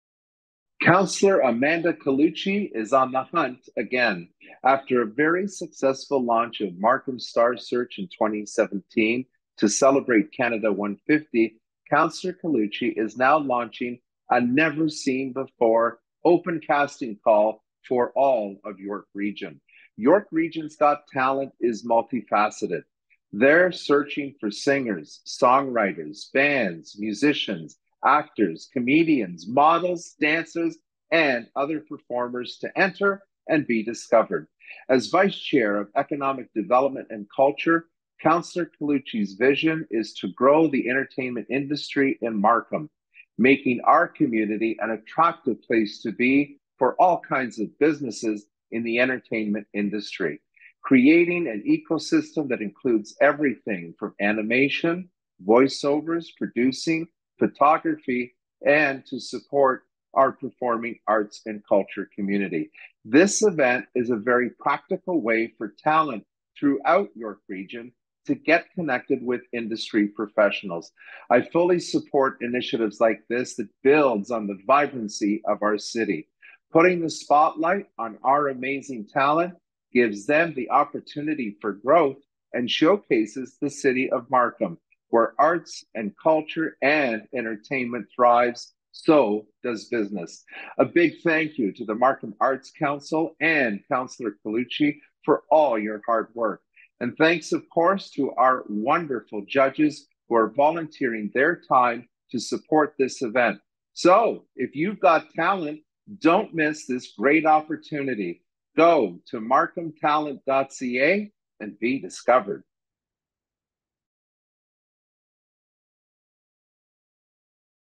Markham Talent Honorary Chair, City of Markham Mayor Frank Scarpitti
Councillor Collucci has done it again!  After a very successful launch of Markham Star Search in 2017 to celebrate Canada 150, Councillor Collucci is now launching a never seen before Open Casting Call for all of York Region in a multi-faceted York Region’s Got Talent search in all categories: Singer/songwriter/band/musician/actor/comedian/model/dancer and other performers to enter and BE DISCOVERED.
As Vice Chair of Economic Development & Culture, Councillor Collucci’s vision is to grow the entertainment industry in Markham, making Markham an attractive place to be for all kinds of entertainment related business, creating an eco-system here locally, including animation, voice over, producing, photography to support our performing arts and culture.  This event is a very practical way for any talents to get connected with industry professionals currently working in the industry.

I fully support any opportunities will bring vibrancy and actions in the city!  Congratulations to Markham Arts Council and Councillor Collucci for all your hard work.  Clearly, we cannot do this without our wonderful team of judge panel who volunteer their time to make this event a reality.  If you’ve got talent, you don’t want to miss this great opportunity, make sure to register!

and BE DISCOVERED!